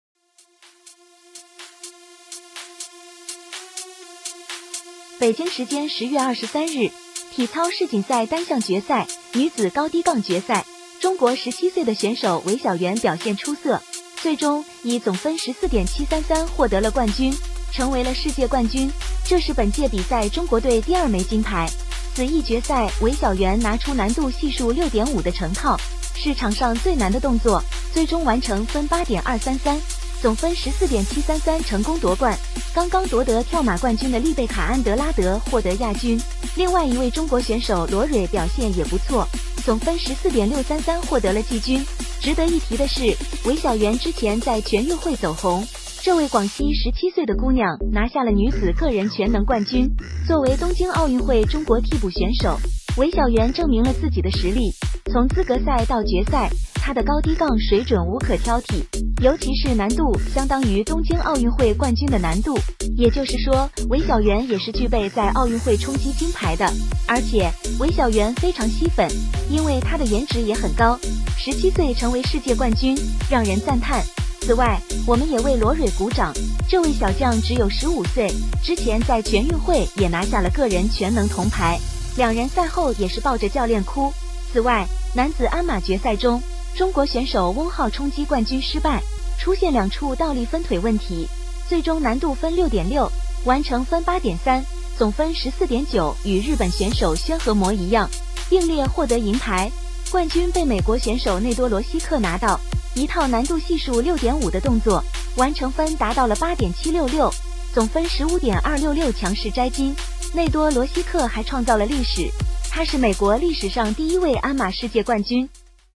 体操世锦赛第二金！中国体操诞生新世界冠军：17岁颜值出众，与教练相拥而泣
北京时间10月23日，体操世锦赛单项决赛，女子高低杠决赛，中国17岁的选手韦筱圆表现出色，最终以总分14.733获得了冠军，成为了世界冠军！这是本届比赛中国队第二枚金牌。